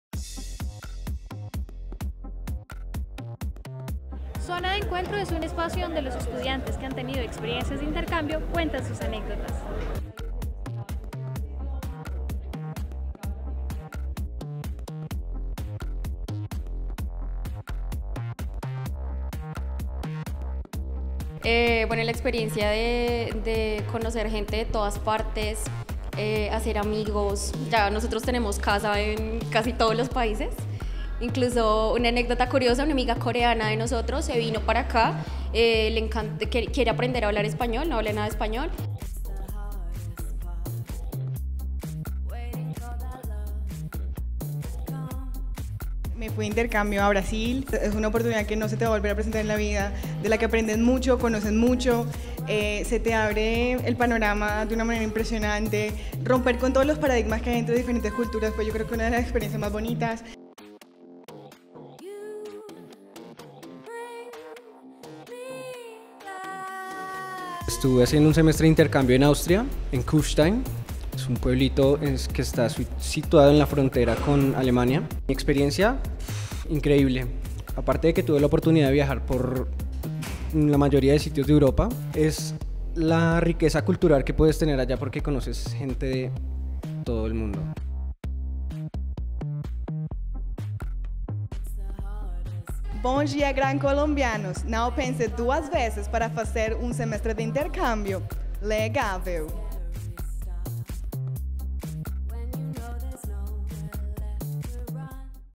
Zona de encuentro estudiantes de intercambio
En el Auditorio Bloque K se convirtió en el centro de reunión para decenas de estudiantes que el semestre pasado  viajaron a diferentes países, a vivir una experiencia académica, cultural y laboral. La zona de encuentro fue organizada por el  Departamento de Movilidad   y  busca promover el intercambio  de estudiantes y docentes, a nivel nacional e internacional. Los asistentes compartieron sus anécdotas y recibieron  sus respectivas certificaciones.